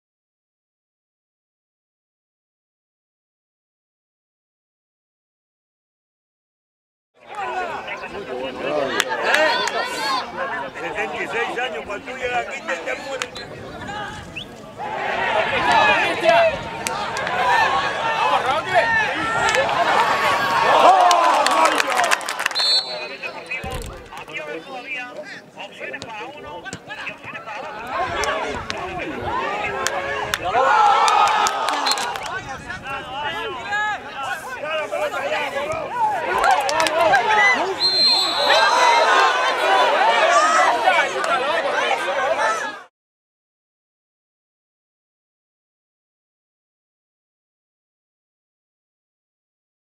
Las Palmas Atlético-Lanzarote
Las Palmas Atlético y el Lanzarote empataron a un gol. Roque marcó para los amarillos, mientras que Echedey lo hizo para el cuadro rojillo.